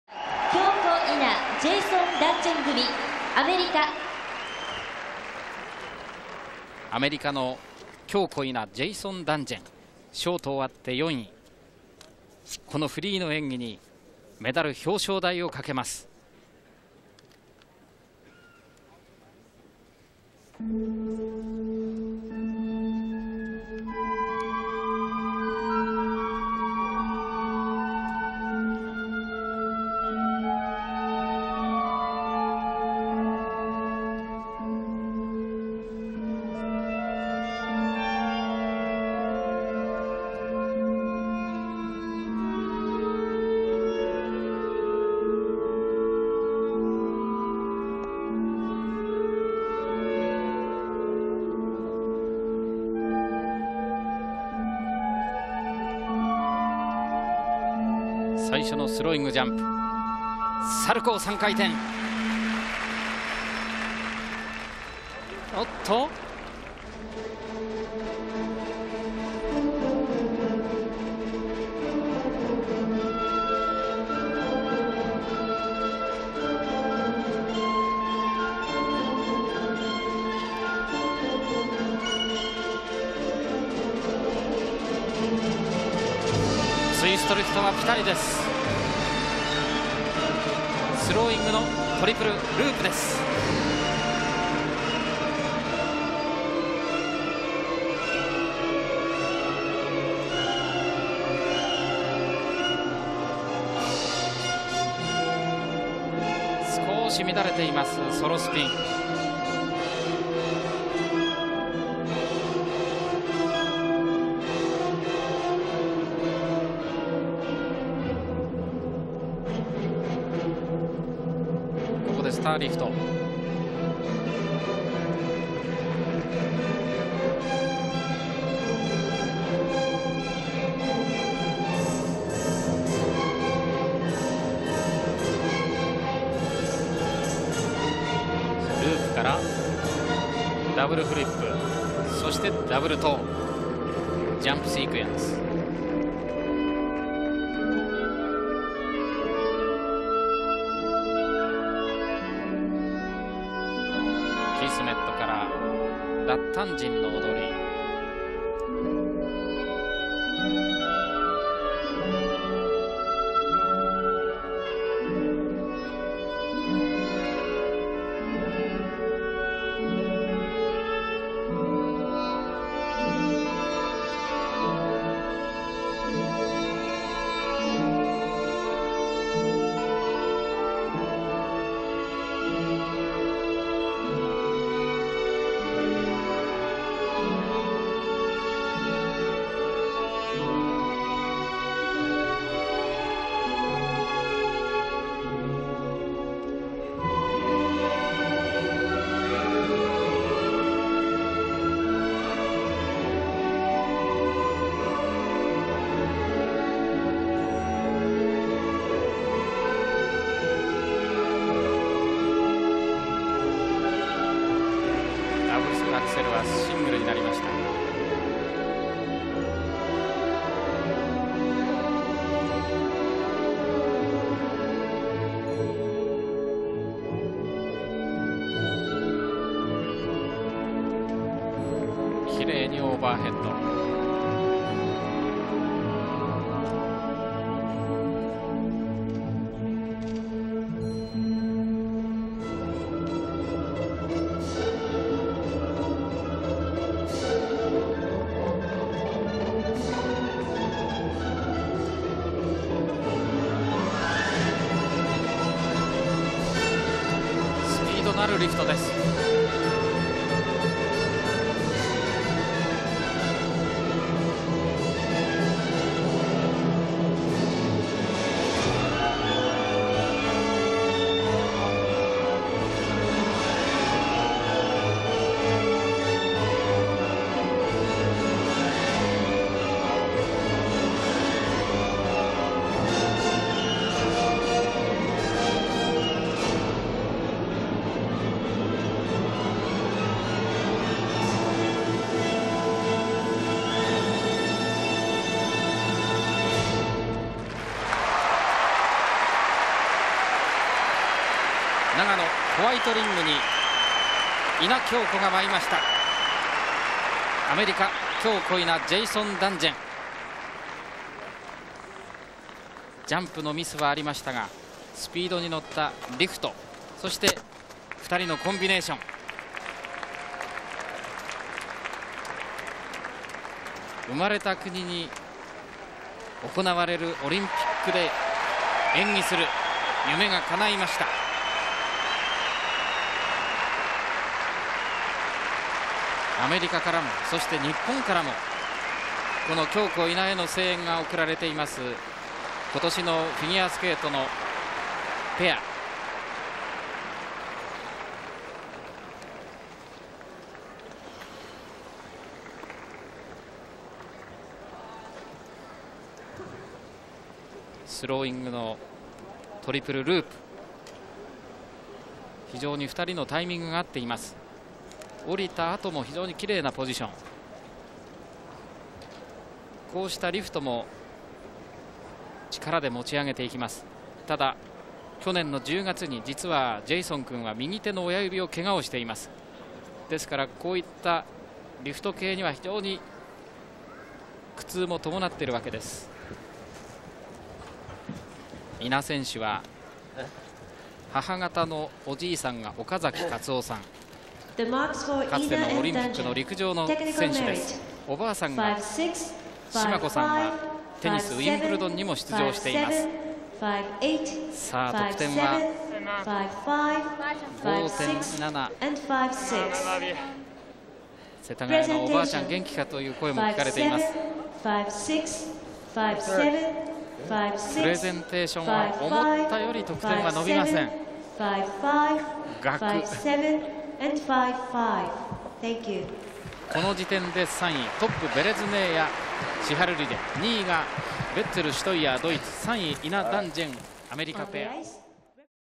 [HD] Ina & Dungjen - 1998 Nagano Olympics - FS
Kyoko Ina / Jason Dungjen
Кёко Ина / Джейсон Данджен  1998 олимпиада в нагано
キョウコ・イナ / ジェイソン・ダンジェン  1998 長野五輪

Nagano Winter Olympic Games Figure Skating Pairs Free Skating
зимние Олимпийские игры в Нагано. Парное катание. Произвольная программа.
長野オリンピック フィギュアスケート ペア フリースケーティング